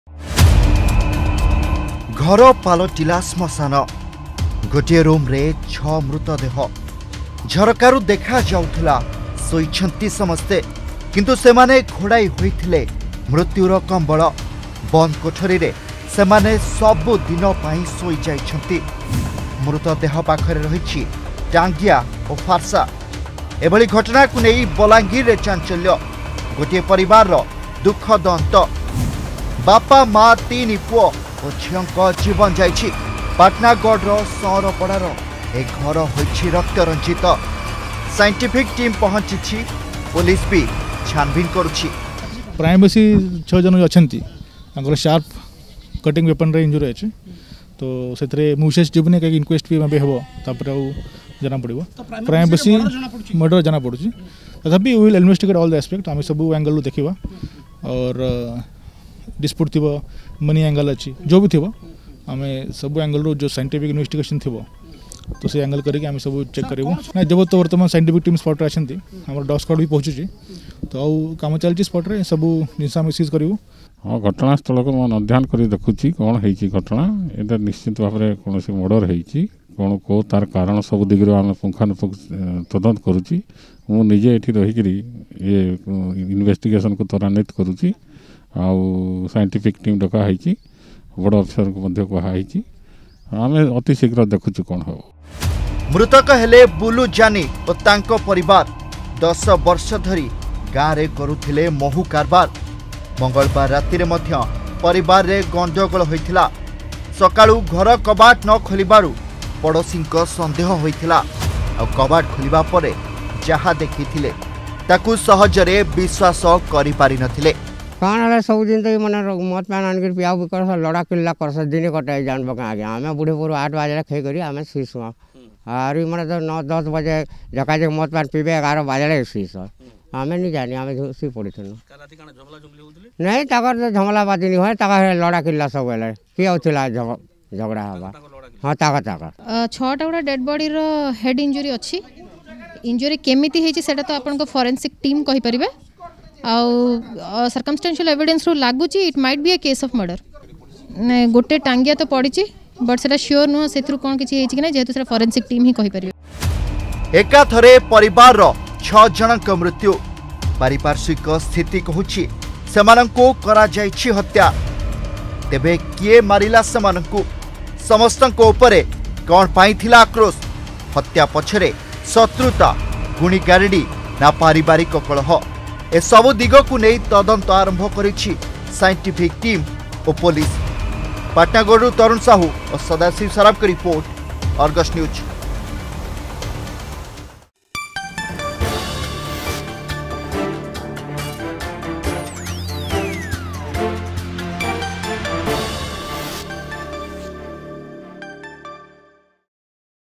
ଘର ପାଲଟିଲା ଶ୍ଣଶାନ
ବନ୍ଦ ଘରେ 6 ମୃତଦେହ । ଏକା ସାଙ୍ଗରେ ନିଶ୍ଚିହ୍ନ ହୋଇଗଲା ଗୋଟିଏ ପରିବାର । କାରଣ କଣ? ଖୋଜୁଛି ପୋଲିସ ଓ ସାଇଣ୍ଟିଫିକ ଟିମ । ବଲାଙ୍ଗୀର ପାଟଣାଗଡ଼ରେ 6 ଜଣଙ୍କ ମୃତ୍ୟୁକୁ ନେଇ ଚାଞ୍ଚଲ୍ୟ । ରକ୍ତରଞ୍ଜିତ ଘରୁ ସୁରାକ ଖୋଜୁଛି ତଦନ୍ତକାରୀ ଟିମ ...